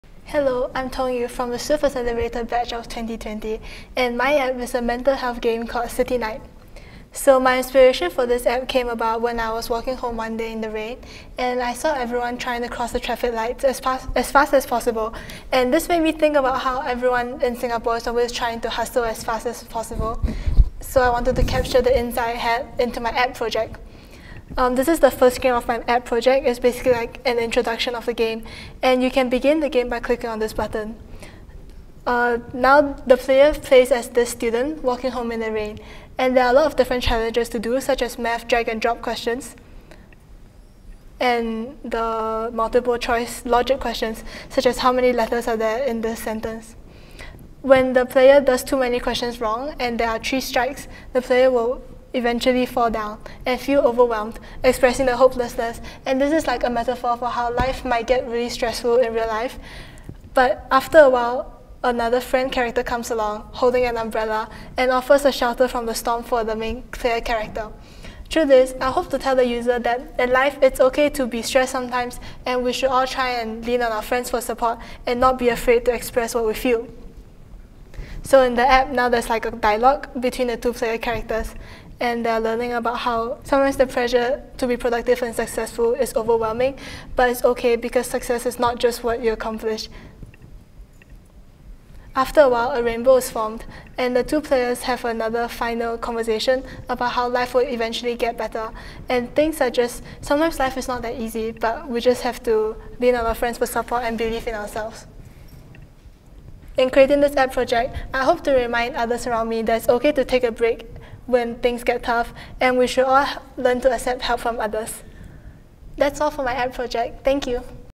Jiang Tongyu - WWDC23 Swift Student Challenge WInner
Learn more about WWDC23 Swift Student Challenge Winner, Tongyu's submission focusing about mental health and slowing down in an otherwise hustling world.